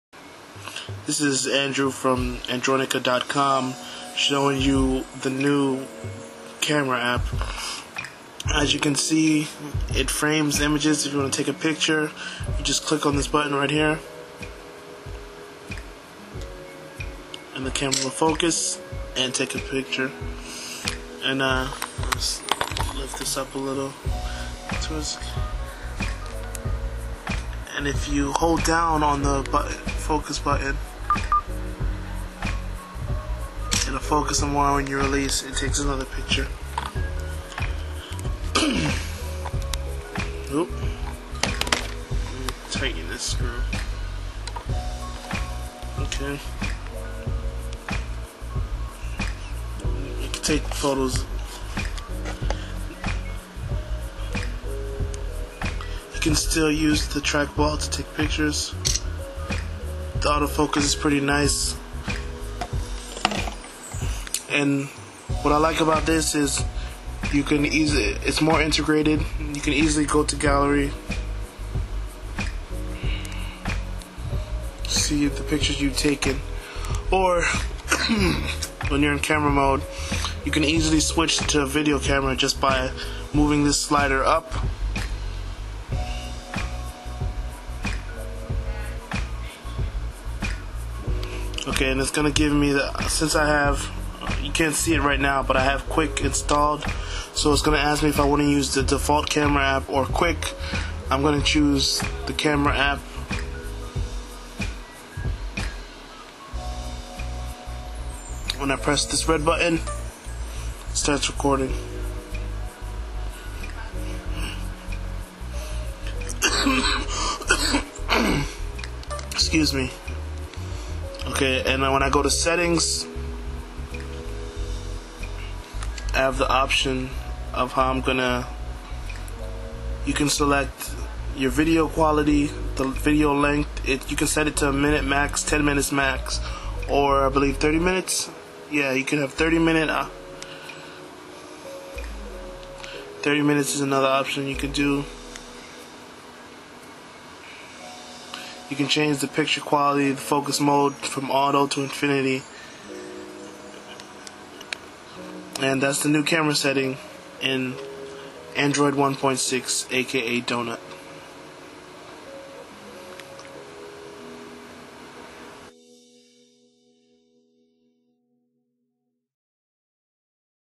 Android Donut;: The Camera/Camcorder Interface
A look at the camera/camcorder interface in the Donut build of Android. Video taken on the Cyanogen Mod that already includes donut. for more Android news, tips, tutorials, videos, and all things Android.